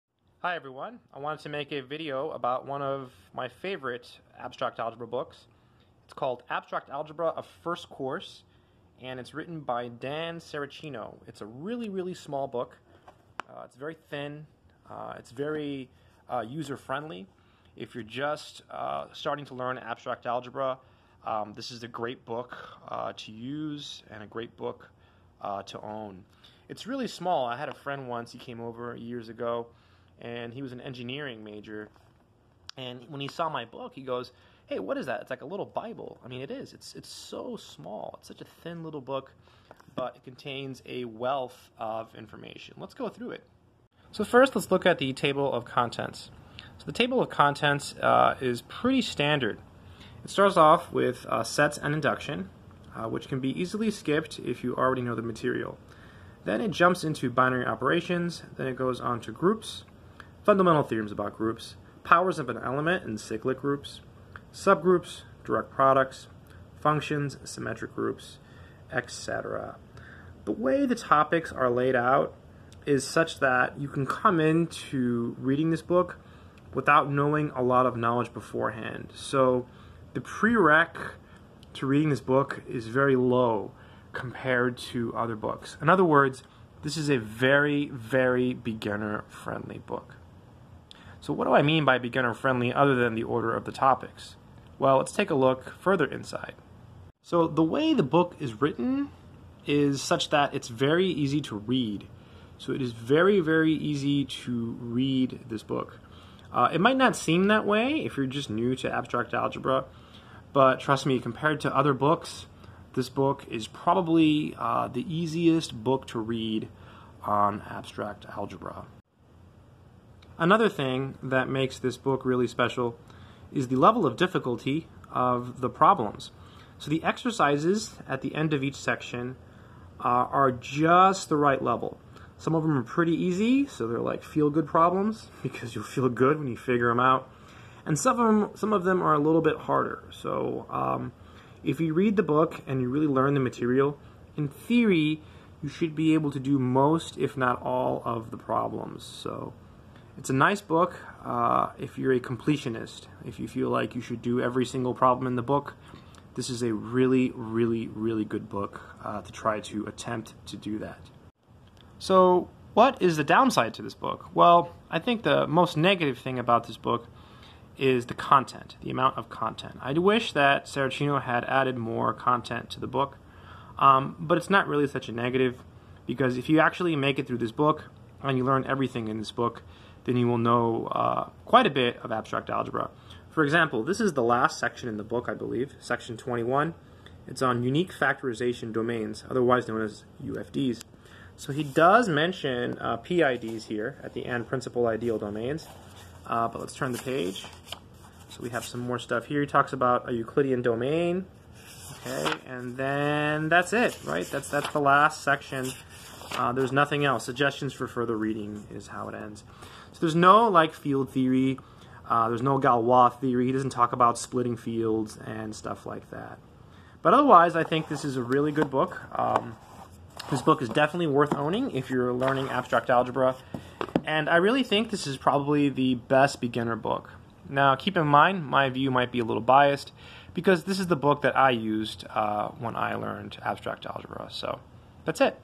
The Best Beginner Book to Learn Abstract Algebra "Abstract Algebra A First Course by Dan Saracino"
This is the book I learned Abstract Algebra from years ago. If I had to pick a personal favorite this would be it. Compared to other books it is very easy to read and I highly recommend it if you are just starting out. In this video I go over the pros and cons of the book.

Link to book on amazon is

I hope you find this video helpful and please leave any questions or comments in the comment section below. Thank you:)